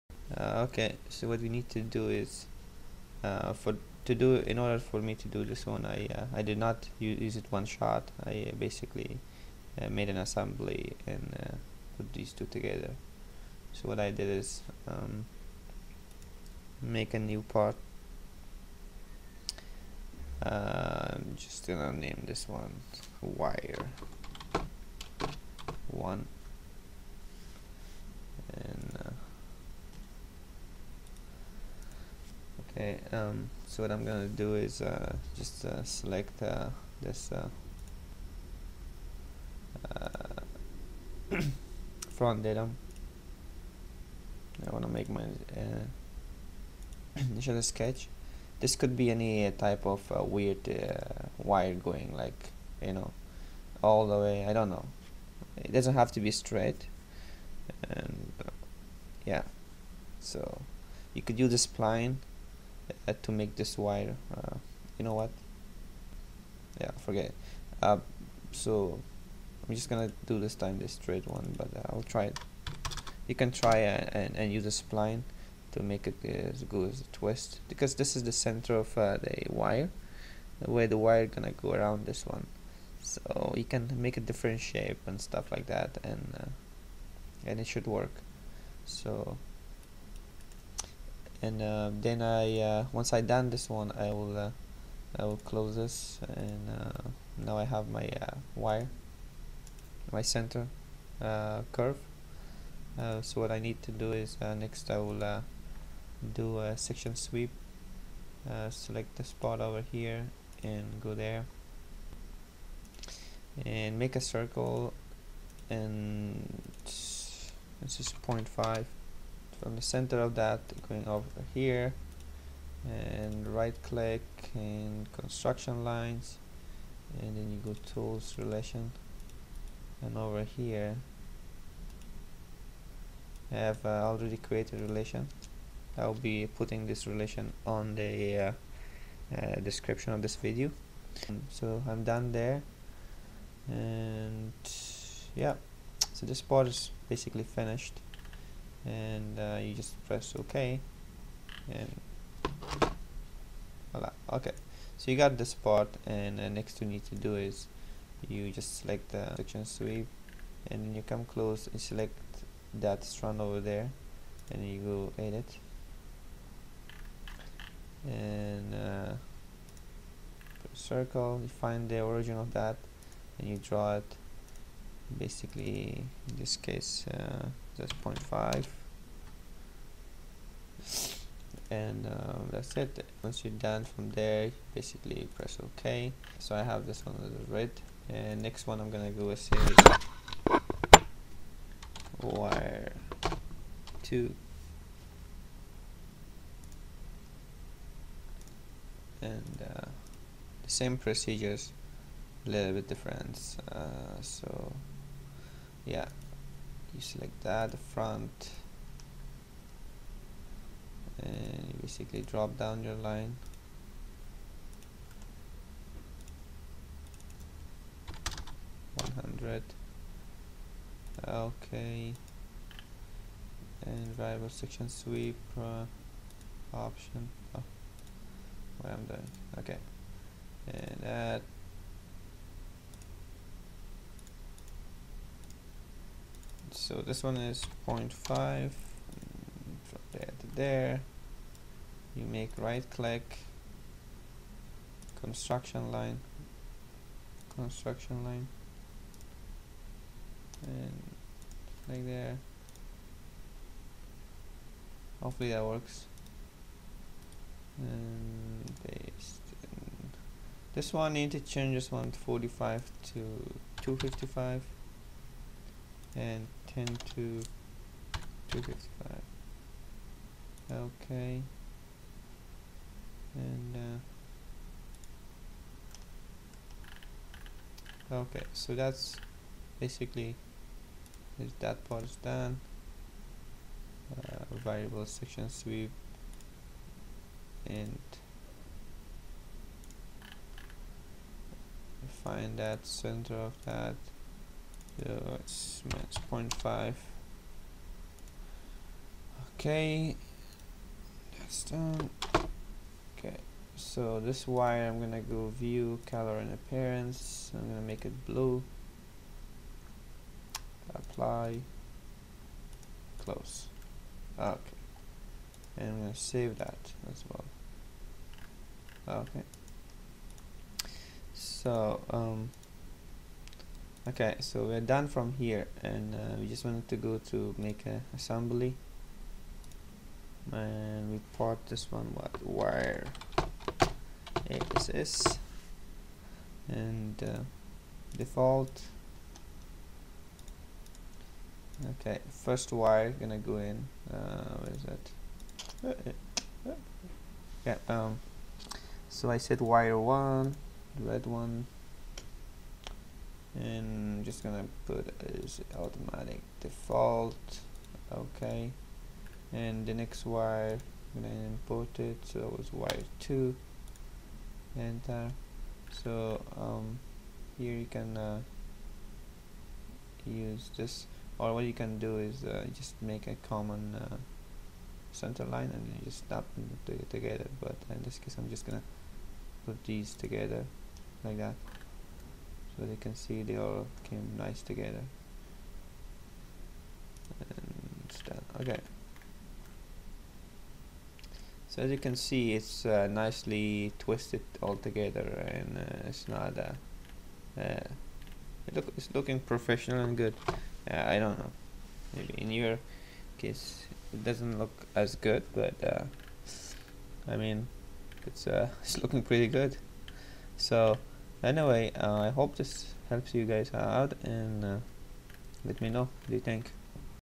Twisted pair of Wire Proe tutorial
In this tutorial i will show you how to make a twisted wire using proe cad software.